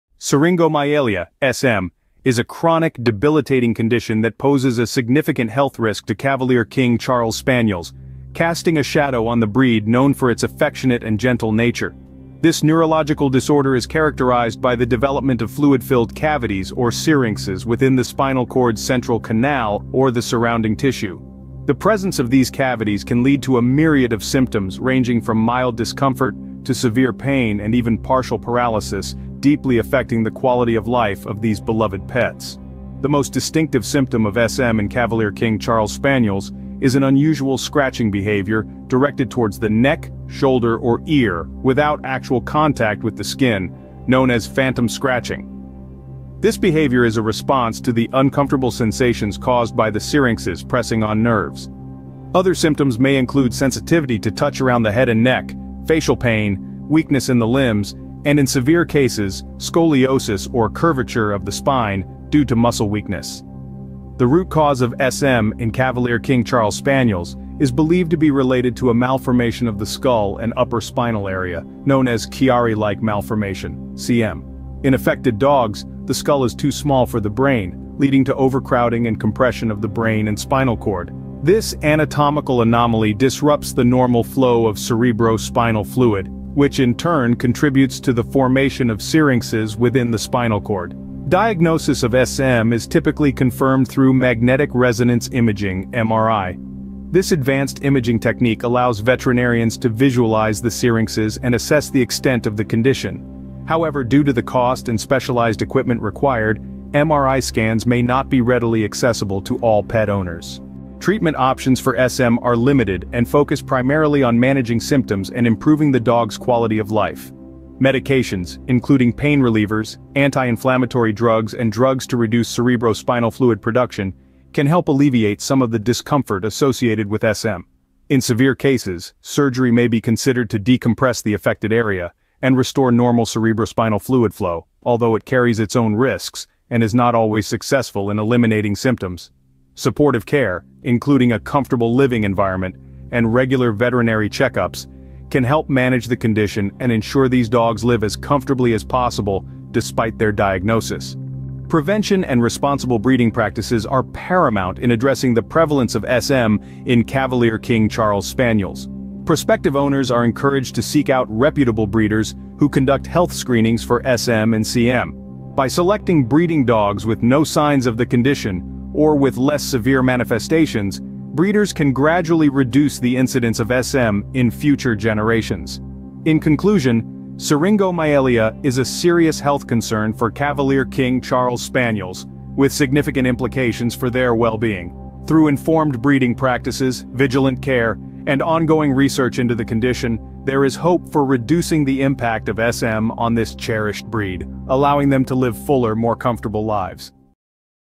Decoding Syringomyelia in Cavaliers | Phantom Scratches
Discover the hidden world of Syringomyelia in Cavalier King Charles Spaniels with "Decoding Syringomyelia in Cavaliers." This eye-opening video delves into the mysterious symptoms and challenges faced by these beloved dogs. Unravel the complexities of this neurological condition, from phantom scratching to severe pain, and learn about the latest treatments and research. Join us in exploring the impact of Syringomyelia on Cavaliers and the hope for a healthier future. Don't miss this intriguing deep dive into their lives.